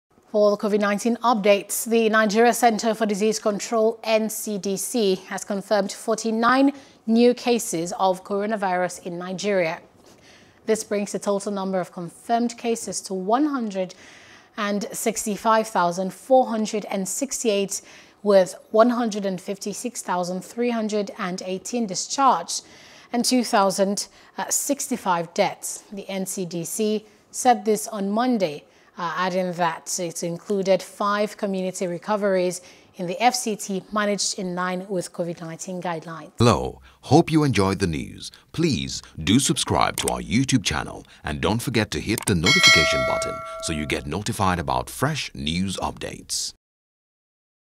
Nigeria Confirms 49 New Cases | CORONAVIRUS
The Nigeria Centre for Disease Control, NCDC has confirmed 49 new cases of Covid-19 bringing the total number of the confirmed cases to 165,468 with 156,318 Discharged and 2,065 Deaths.